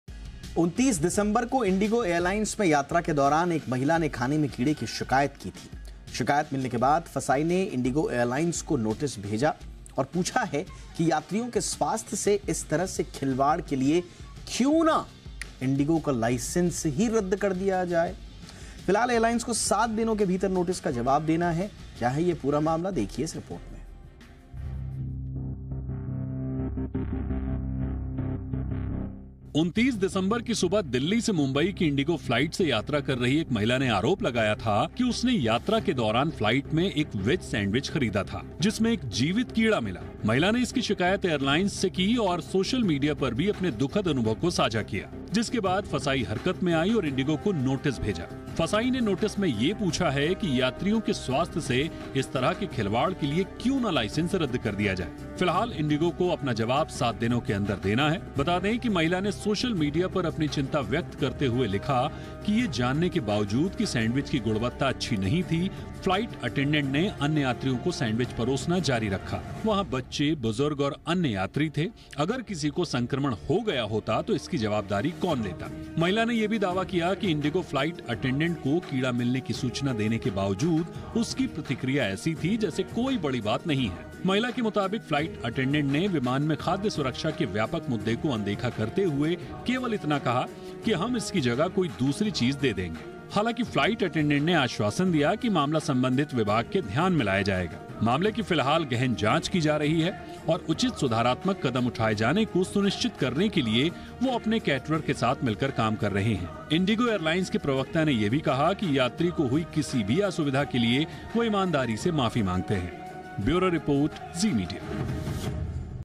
‘Worm in sandwich:’ FSSAI notice to IndiGo over ‘unsafe for human consumption’ food
Indigo की फ्लाइट में सफर कर रही एक महिला यात्री के सैंडविच में कीड़ा मिलने के बाद फूड सेफ्टी रेगुलेटर ने इंडिगो को नोटिस जारी किया